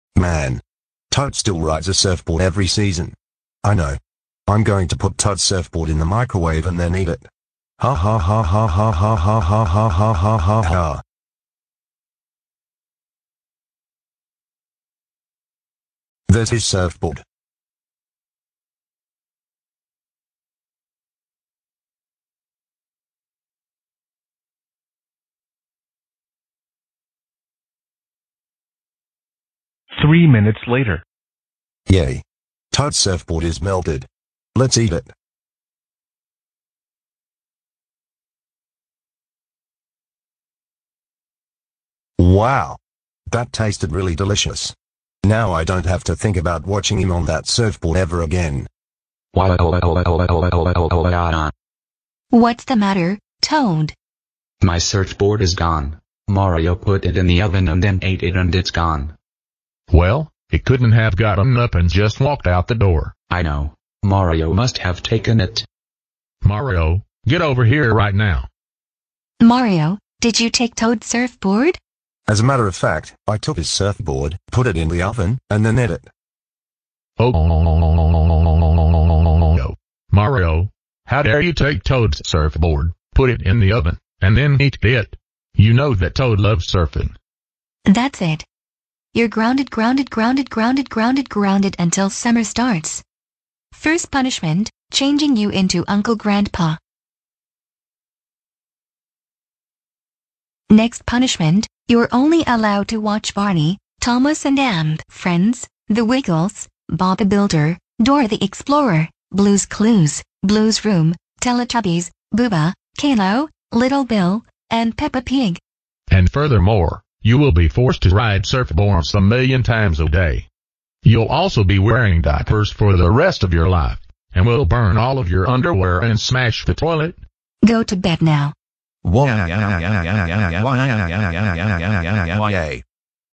Mario Puts Toad's Surfboard in the Oven and Gets Grounded
Inspired By: MattTheGoAnimator's "Arthur Gets Grounded" Series